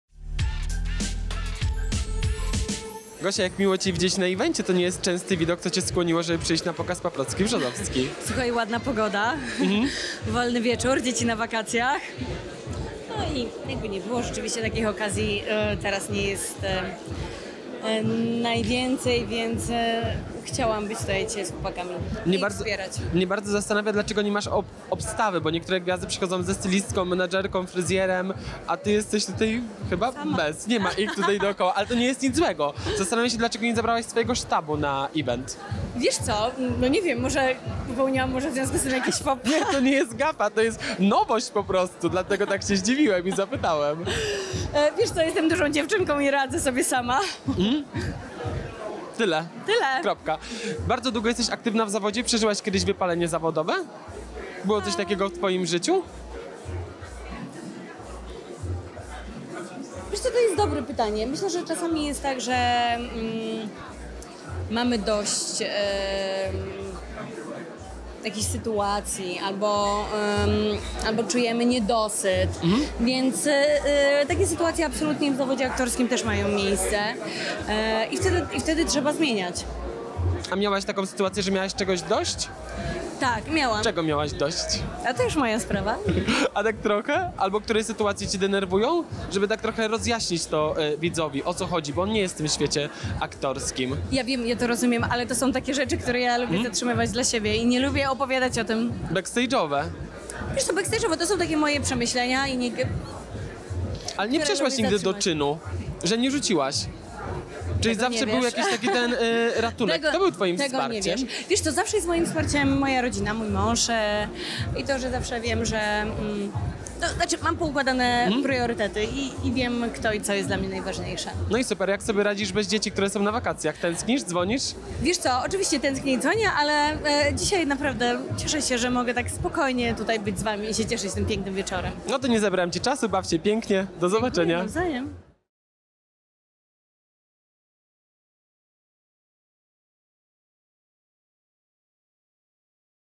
Małgorzata Socha wbiła szpile gwiazdom! Przeżyła wypalenie zawodowe?
Małgorzata Socha wywiad.